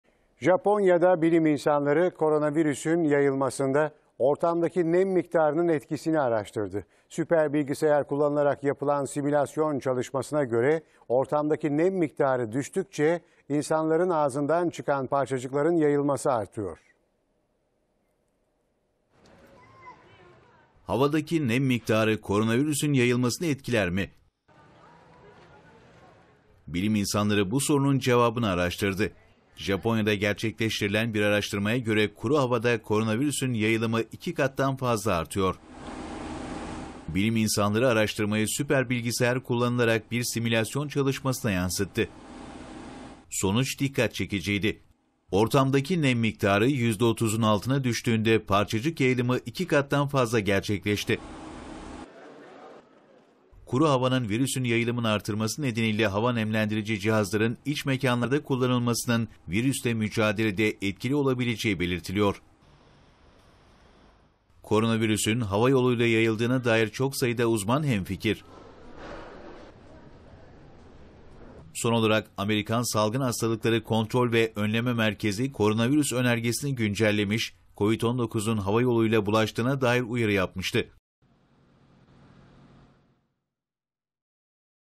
Virüs Kuru Havada 2 Kat Fazla mı Yayılıyor? / A Haber
2019 yılının Aralık ayında Çin'in Wuhan kentinde ortaya çıkan ve kısa süre içerisinde dünyayı etkisi altına alan koronavirüs salgınıyla ilgili şoke eden bir açıklama yapıldı.